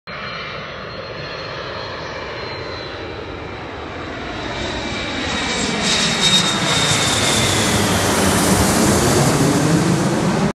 World Atlantic Airlines MD-83 Landing at Tampa International Airport (KTPA)
This was filmed on 1/3/21. World Atlantic Airlines MD-83 landing at Tampa International Airport. Video from Cypress Point Park.